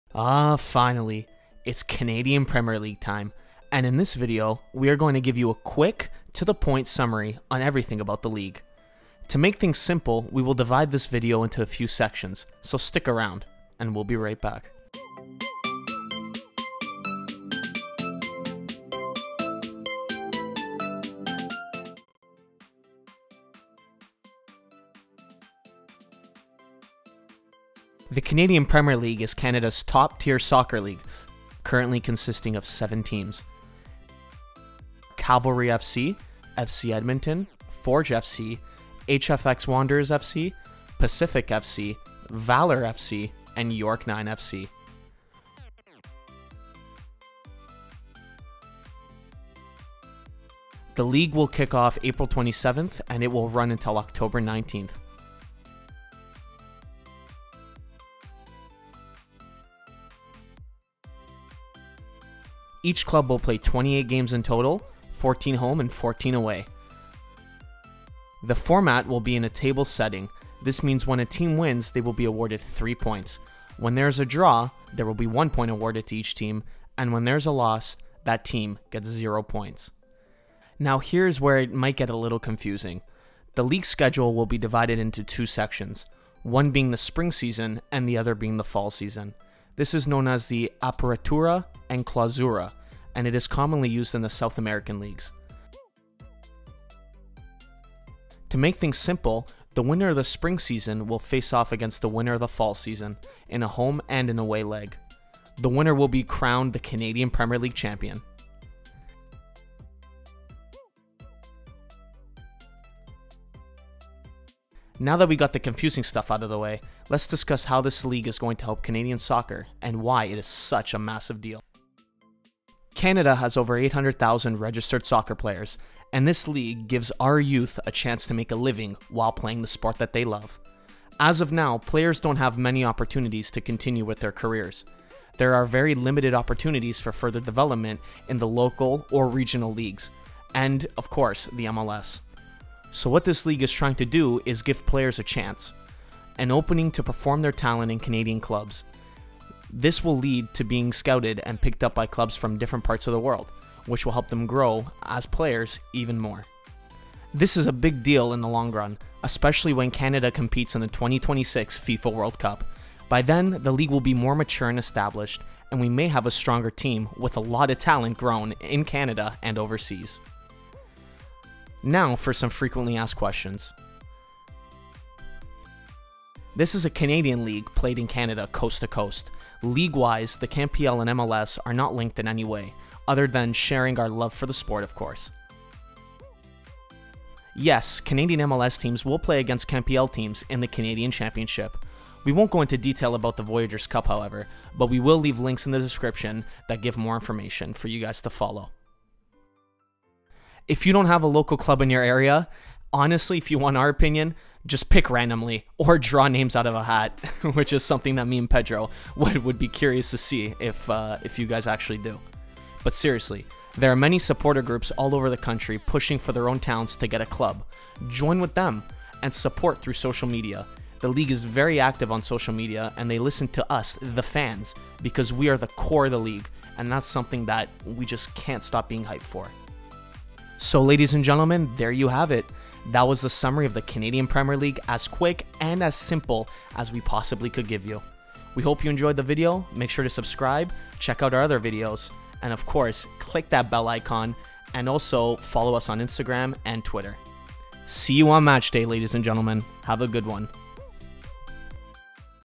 CANADIAN PREMIER LEAGUE EXPLAINED IN 5 MINUTES
Ever try the Canadian Premier League to somebody?  Are you tired of having to explain the same thing over and over and over again? Well say no more! You can use this video which sums up (most of) the league, to show everyone!